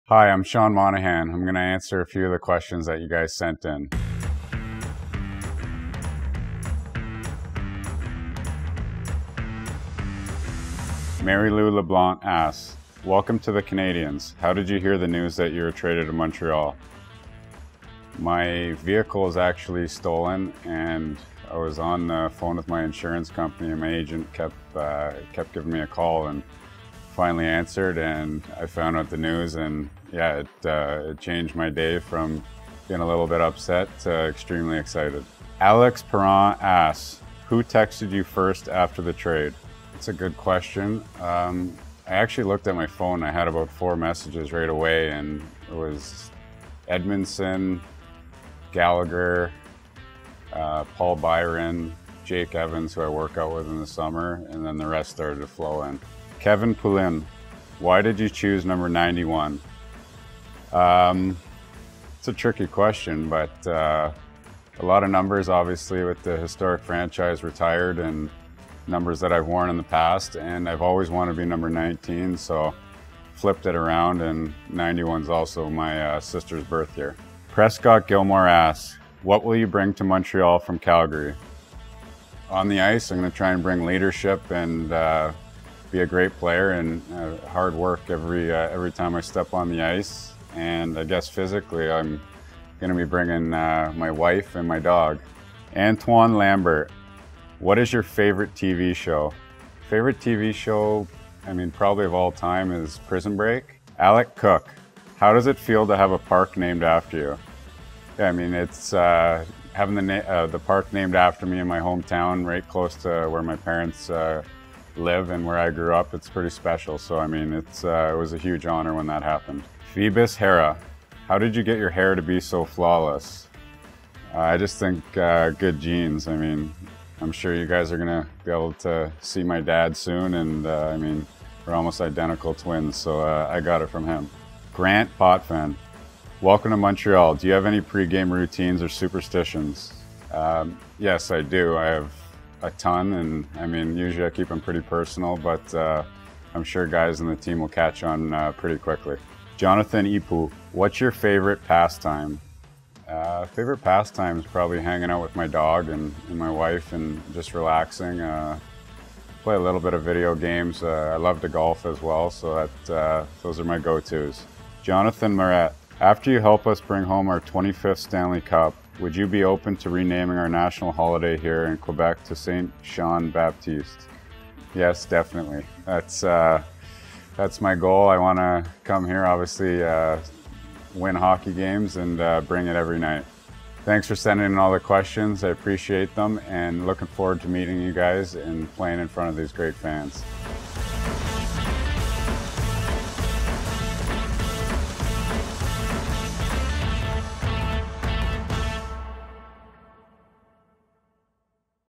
Sean Monahan answers fan questions | Ask a Hab
Recently acquired from the Calgary Flames, Sean Monahan answers fan questions submitted to the canadiens.com mailbag.